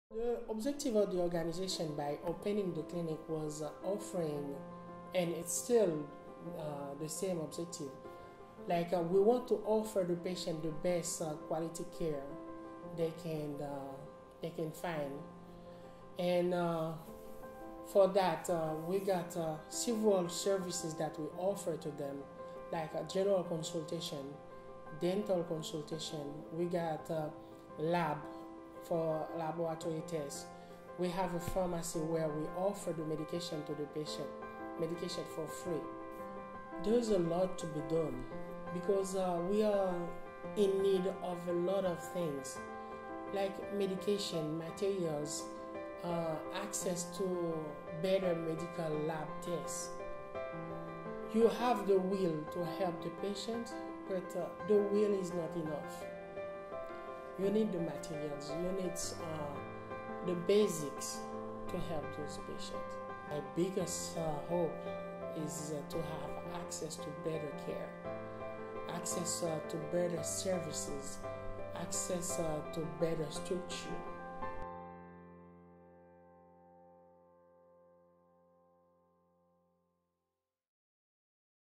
Our Biggest Hope: Healthcare
Learn about Hope for Haiti's Healthcare Program and learn what Community Health Manager Dr. Steeve Victor's biggest hope for Haiti is!

Video by: Wiley Parker, 2016